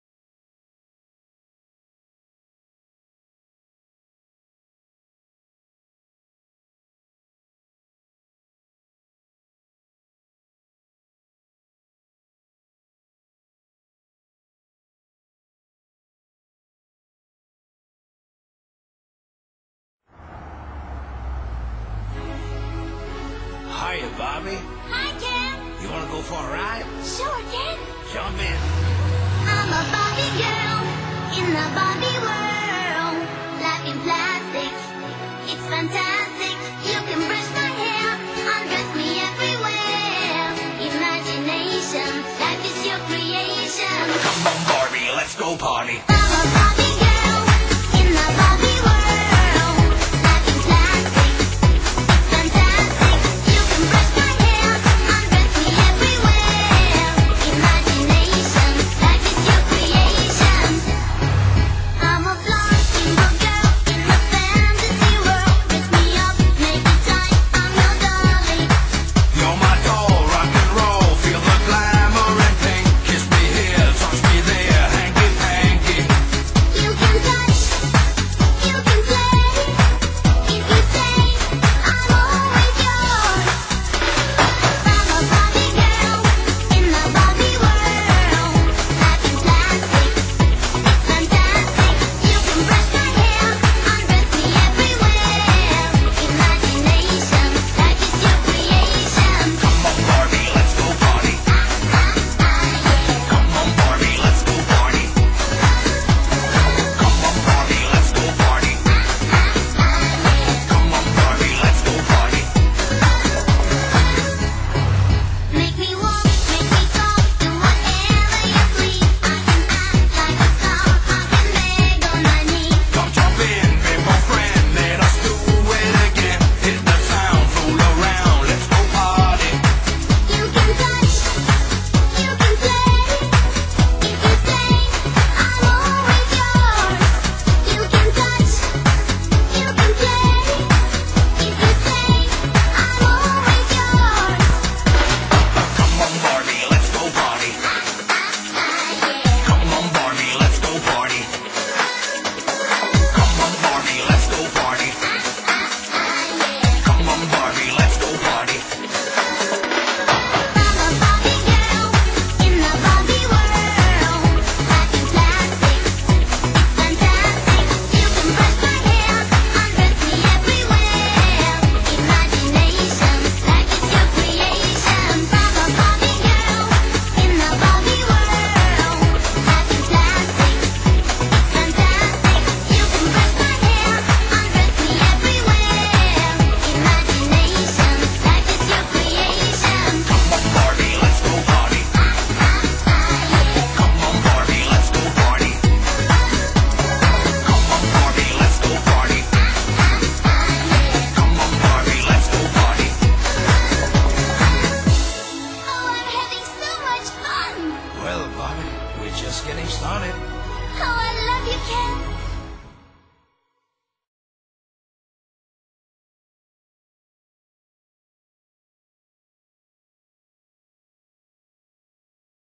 Transformer-Girl
Kaze~Neko Productions Presents Transformer-Girl; A silly, dumb Anime Music Video inspired by a friend who likes Transformers and Aqua.  I blatently admit that this video sucks, and you don't need to tell me again, but it's funny if you possess the right mind-frame.